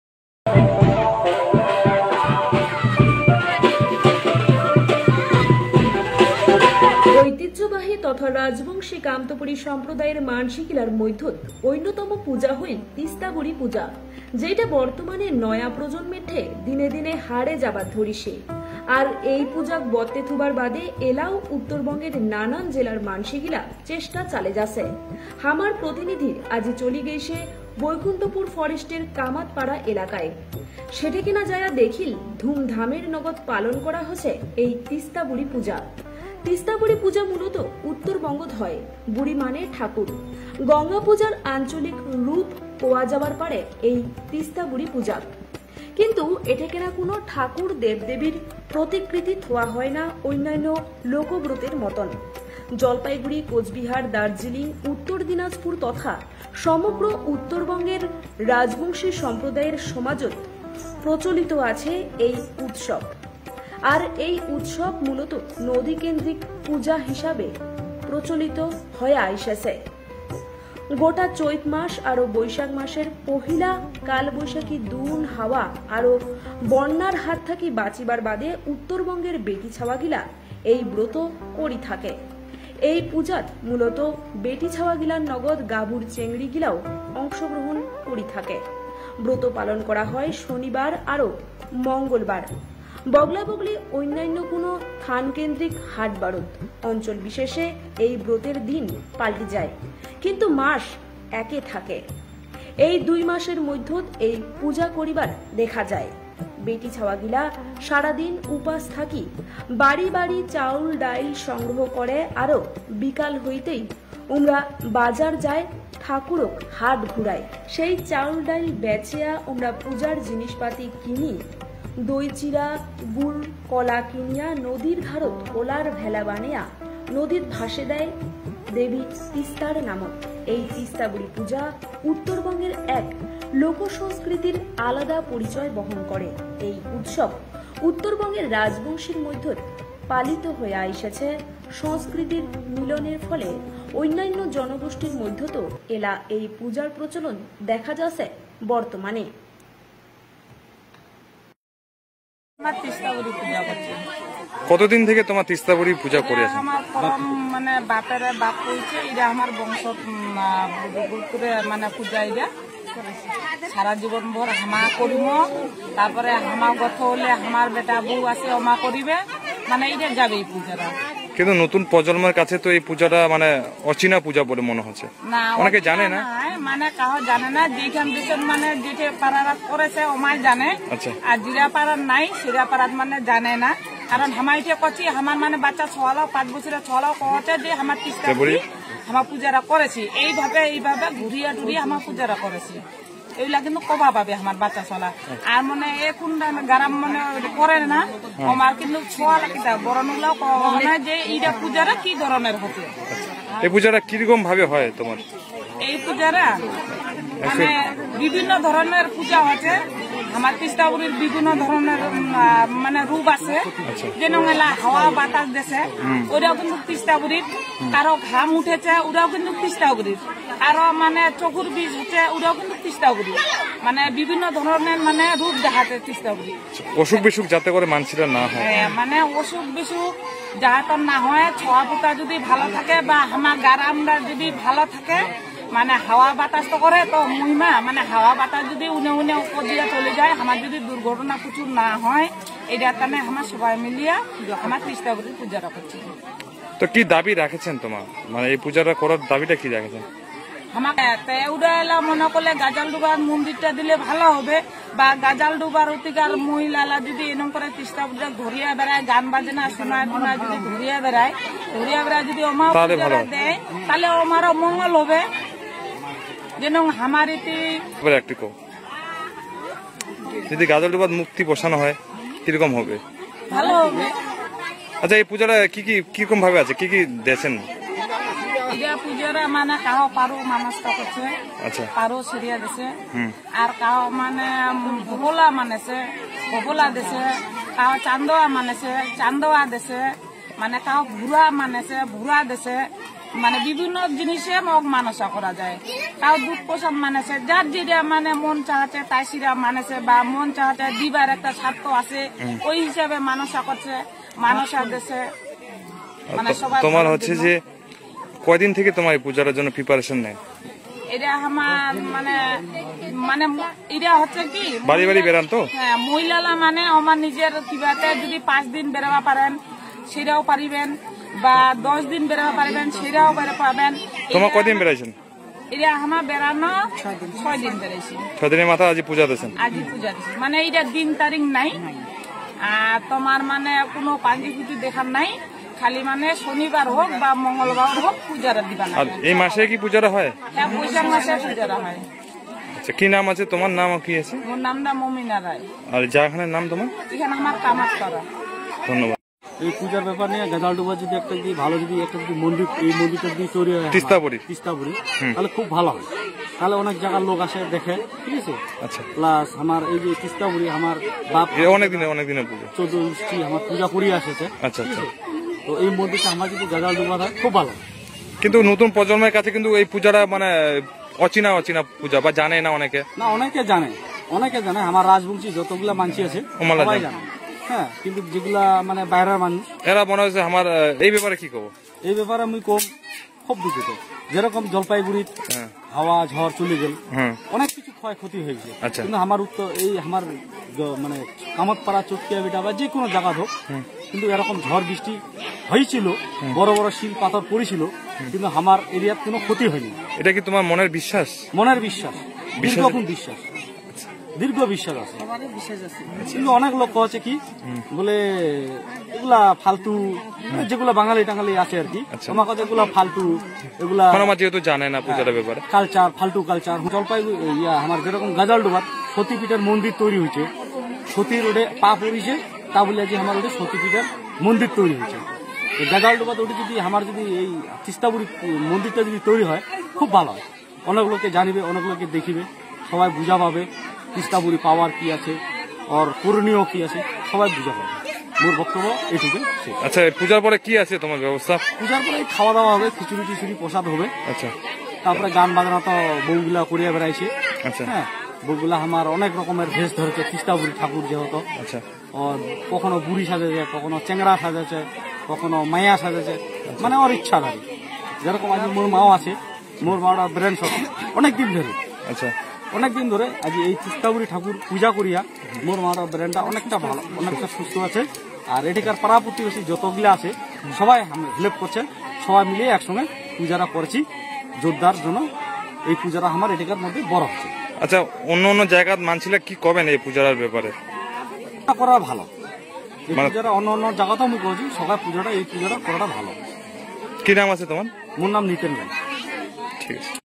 ♦️♦️ তিস্তাবুড়ি পুজাক বত্তে থুবার চেষ্ঠা করেছে বেটিছাওয়ালা♦️♦️কেনে এই তিস্তাবুড়ি পুজা করা হয়
♦️♦️ তিস্তাবুড়ি পুজাক বত্তে থুবার চেষ্ঠা করেছে বেটিছাওয়ালা
♦️♦️কেনে এই  তিস্তাবুড়ি পুজা করা হয়